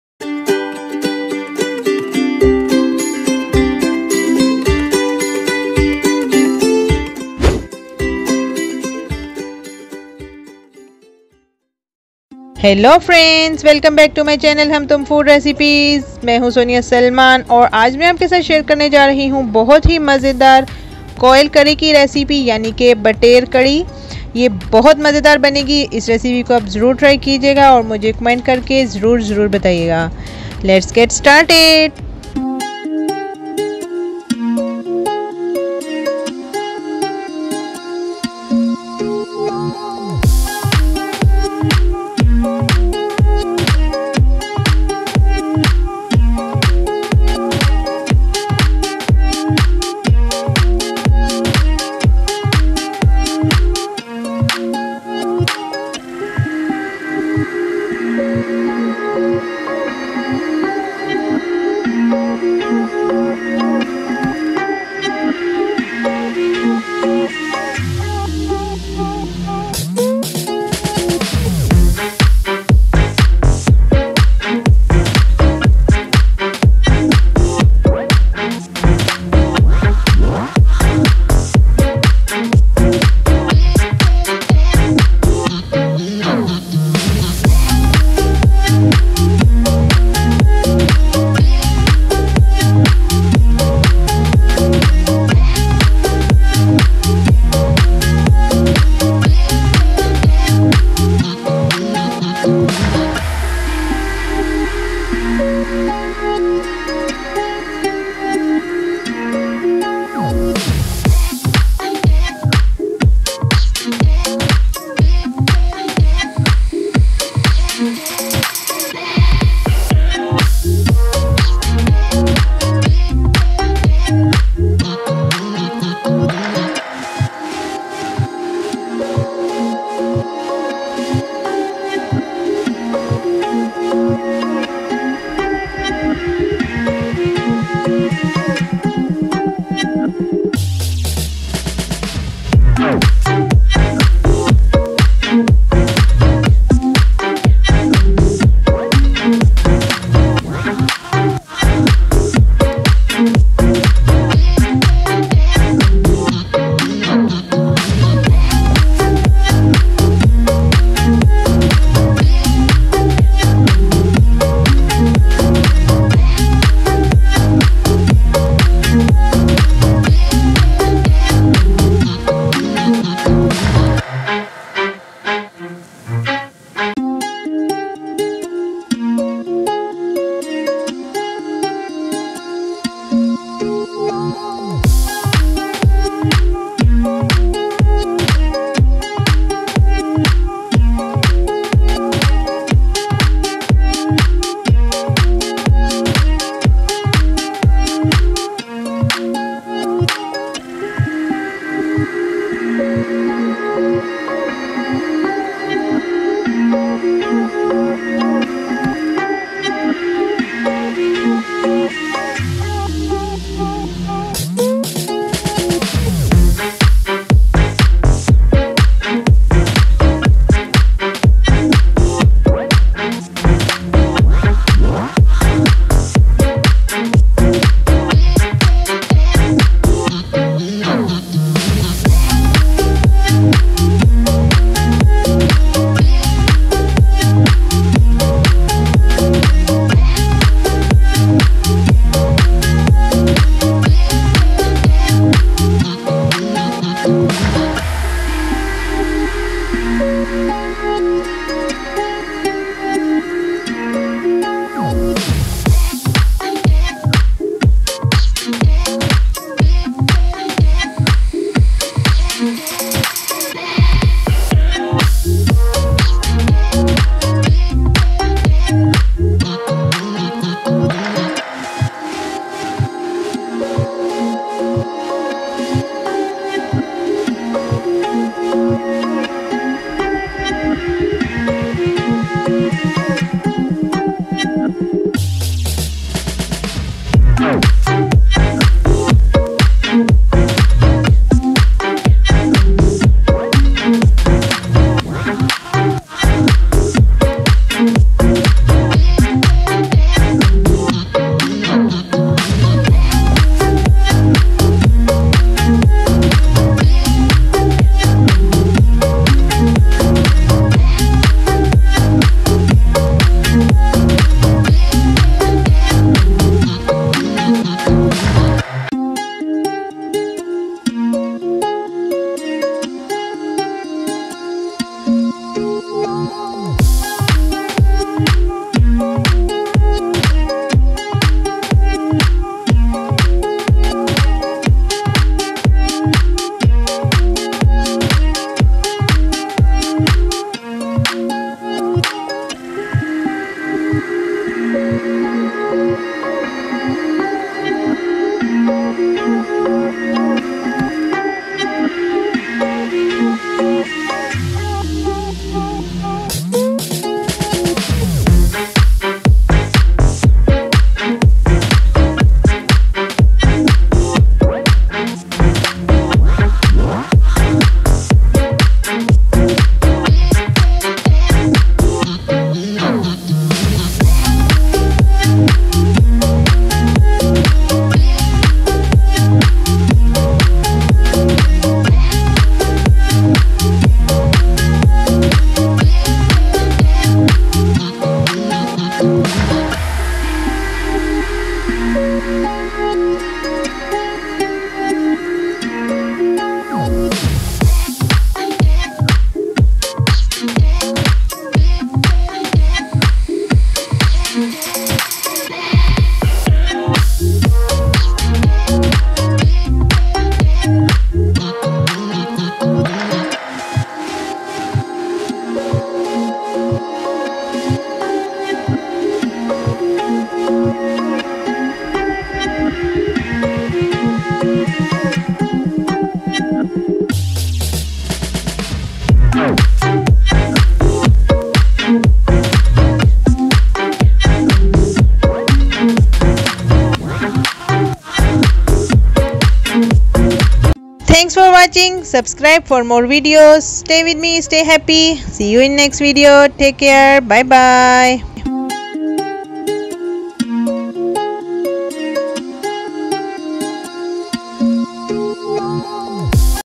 How To Make Tasty Quail Curry | Batair ka Salan Restaurant Styal | Batair Recipe | Bater Curry
How To Make Tasty Quail Curry | Batair ka Salan Restaurant Styal | Batair Recipe | Bater Curry by Ham Tum Foods Recipes

Friends in this video I will show you how can you make Batair curry at home.Quail is very healthy .
Quail meat is much better than chicken.
Quail Meat is considered a Valuable
 dietary food.
Quail Meat is very useful.
I cook with Gravy  it can servr with Rice. I use very less ingredients to make this.

www,Facebook.com